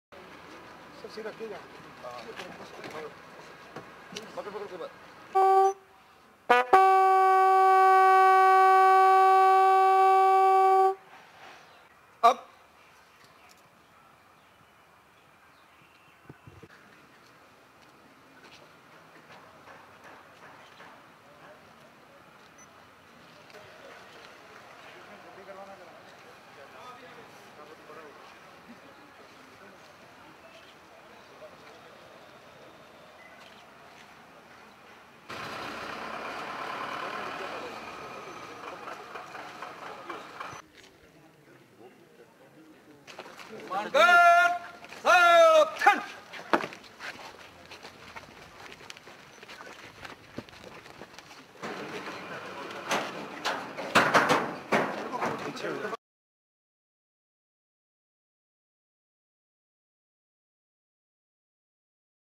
Tributes paid to Traffic cop crushed to death by truck
| Excelsior News |